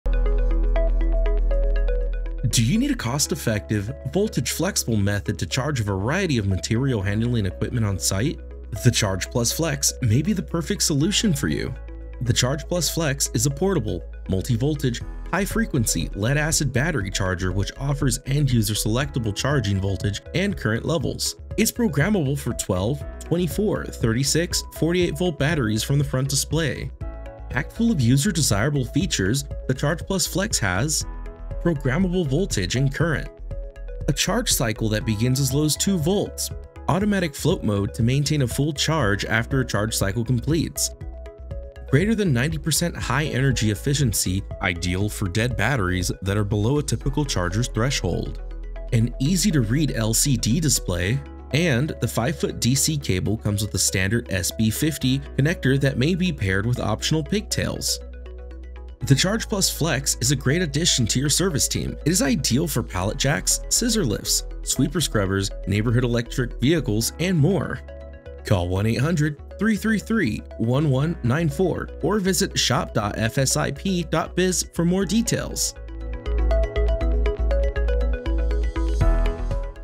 ChargePlus FLEX
The ChargePlus FLEX is a cost-effective solution for your business that can adjust voltage, current, and charging profile through the front touchscreen interface. Charge light duty equipment such as pallet jacks, scissor lifts, aerial lifts, sweeper-scrubbers, and more. This portable, high-frequency charger is adjustable to 12, 24, 36, and 48 volts via display and is programmed for lead-acid batteries. It starts charging with as little as 2-volts and features a float mode to maintain a full charge.